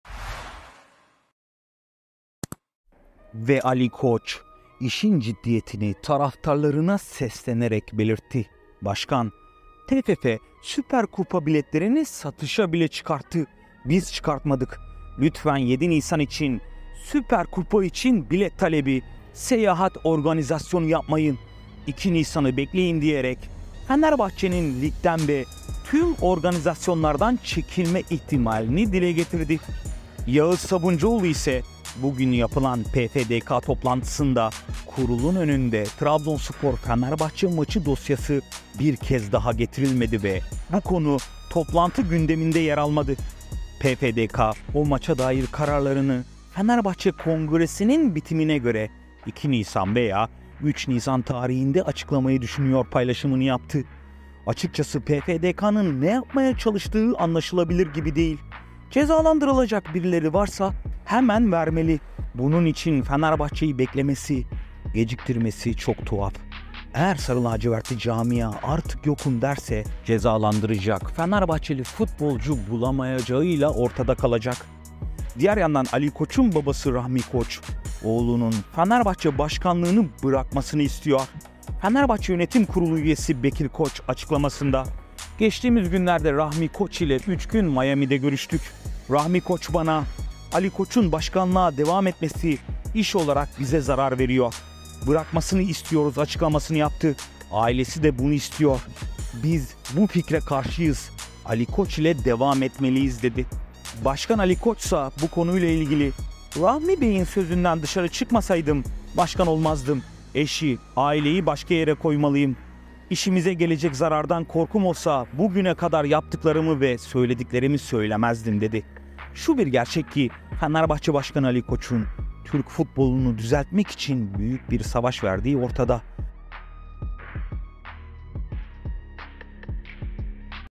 Kupayı reddetti Ligden çekildi || PFDK cezası Fenerbahçe'de
ve ali koç işin ciddiyetini taraftarlara seslenerek belirtti başkan

TFF, Süper Kupa biletlerini satışa bile  çıkardı. Biz çıkarmadık. Lütfen 7 Nisan için  süper kupa için  bilet talebi, seyahat organizasyonu yapmayın. 2 Nisan'ı bekleyin. diyerek fenerbahçenin ligden ve tüm organizasyonlardan çekilme ihtimalini dile getirdi..

yağız sabuncuoğlu ise

 Bugün yapılan PFDK toplantısında kurulun önüne Trabzonspor-Fenerbahçe maçı dosyası bir kez daha getirilmedi ve konu toplantı gündeminde bile yer almadı.
 PFDK o maça dair kararlarını Fenerbahçe kongresinin bitimine göre 2 Nisan veya 3 Nisan tarihinde açıklamayı düşünüyor. paylaşımını yaptı... açıkcası pfdkanın ne yapmaya çalıştığı anlaşılabilir gibi değil cezalandırılacak birileri varsa hemen vermeli bunun için fenerbahçeyi beklemesi geciktirmesi çok tuhaf eğer sarı lacivertli camia artık yokum derse cezalandıracak fenerli futbolcu bulamayacağıyla orta da kalacak...

diğer yandan ali koçun babası rahmi koç oğlunun fener başkanlığını bırakmasını istiyor fenerbahçe yönetim kurulu üyesi bekir koç açıklamasında..

geçtiğimiz günlerde Rahmi Koç ile 3 gün Miami'de görüştük. Rahmi Koç  bana "Ali Koç'un başkanlığa devam etmesi, iş olarak bize zarar veriyor. Bırakmasını istiyoruz." açıklamasını yaptı. Ailesi de bunu istiyor. Biz bu fikre karşıyız. Ali Koç ile devam etmeliyiz." dedi

başkan ali koçsa bu konuyla ilgili..
Rahmi Bey'in sözünden dışarı çıkmasaydım başkan olmazdım. Eşi, aileyi başka yere katmayalım. İşimize gelecek zarardan korkum olsa, bugüne kadar yaptıklarımı ve söylediklerimi söylemezdim." dedi

şu bir gerçek ki fenerbahçe başkanı ali koçun türk futbolunu düzeltmek için büyük bir savaş verdiği ortada öyleki iş hayatını da etkiliyor ancak tüm branşlarda başarılar elde ederken basketbolda avrupa fenerbahçeyi konuşurken voleybolda kaldırılmadık kupa kalmamaşıken futbolda başarı ve kupa gelmeyince pek bir anlamı kalmıyor gibi o yüzden eğer bu sene de kupa kazanılmazsa belki de ali koçun bırakması en doğru karar olabilir..